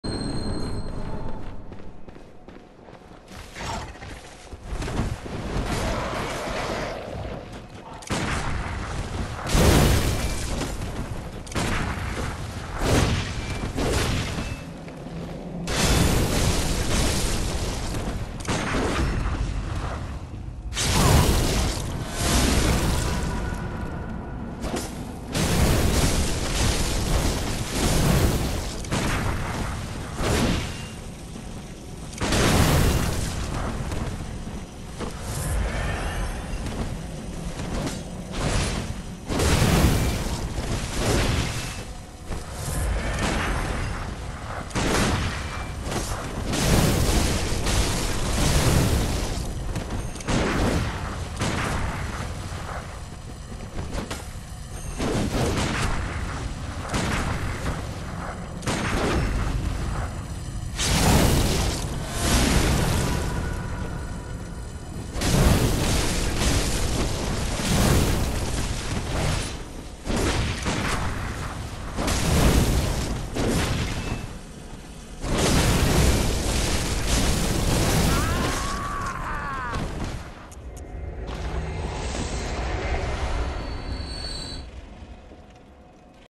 The older Madaras brother
Not a boss, but he put up a huge fight. During my first play through